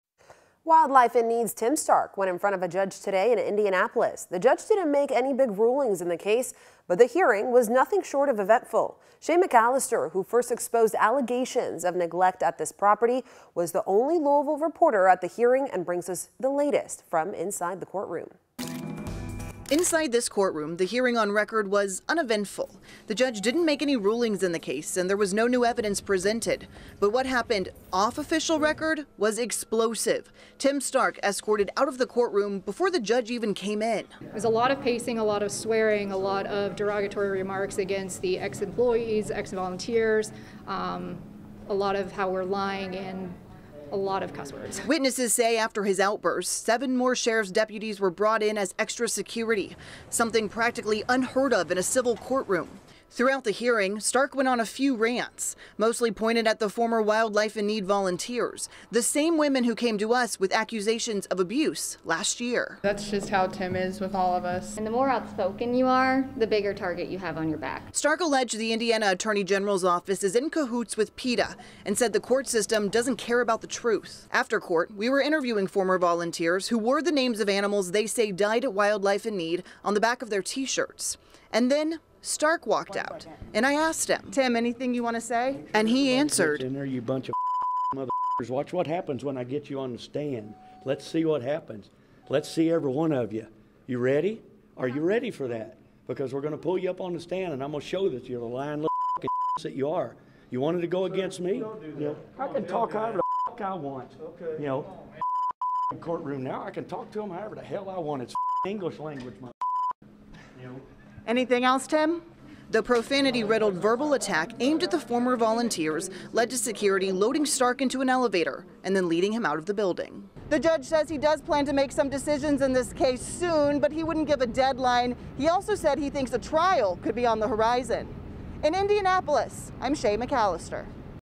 FOCUS: Owner of Wildlife in Need appears in court again
INDIANAPOLIS — Wildlife in Need founder Tim Stark appeared in front of a judge Friday, in his case involving the Indiana Attorney General. The AG's office wants to shut down the non-profit for good, claiming Stark has embezzled money and neglected animals.

The hearing was set to address multiple motions filed in the last month, including the state's request to remove the animals from the property and the judge's ruling that Stark is in contempt of court.

Inside the courtroom, the hearing on record was uneventful.  The judge didn't make any rulings in the case and there was no new evidence presented. But what happened off official record was explosive. Stark was escorted out of the courtroom before the judge even came in, after witnesses said he verbally harassed former employees and volunteers who were there to watch the hearing.

"It was a lot of pacing, a lot of swearing, a lot of derogatory remarks to the ex volunteers about how we're lying and a lot of cuss words," former volunteer Carly Briganan said.

After Stark's outburst, seven more sheriff's deputies were brought in as extra security which is something practically unheard of in a civil courtroom. The officers stood around the perimeter of the room and at the doors.

Throughout the hearing Stark went on a few rants, which were mostly pointed at the former Wildlife in Need volunteers. He accused of spreading misinformation, lying about their time at the facility and working in conspiracy against him.

Former employee Kira Burris explained, "That's just how Tim is with all of us, he doesn't want any of us to tell the real truth. The more outspoken you are the bigger target you have on your back."

Stark also alleged in court that the Indiana Attorney General's Office is in cahoots with PETA and said the court system doesn't care about the truth.

After court, as WHAS11 was interviewing former volunteers, Stark walked out and had a series of profanity riddled words for the group. The verbal attack lead to security loading Stark into an elevator and leading him out of the building. A different set of deputies accompanied the former staff out of a different door.

The judge aid he does plan to make some decisions in this case soon but he wouldn't give a deadline. He also said he thinks a trial could be on the horizon.